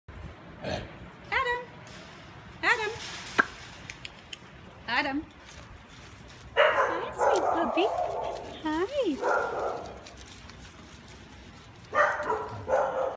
Adopted! Adam adopted!! Adam for adoption
Who doesn’t love puppies, especially one as sweet as our Adam. He is a Labrador mix just five months old and weighing almost 33 pounds. He will be a medium sized adult. Adam enjoys playtime with his siblings and is calm and submissive. He is a gentle and lovable pup and just wants to cuddle up with his people. Rescued by our sister organization in Mexico, Adam has been neutered, dewormed and vaccinated.